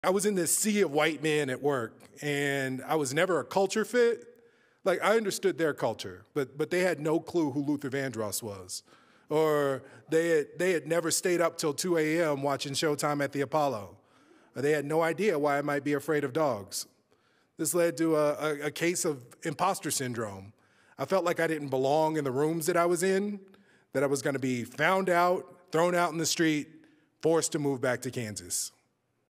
Ivan McLellan | Finding My Place | Story Preview
In his story, Ivan McClellan searches for community and discovers it in an unexpected place.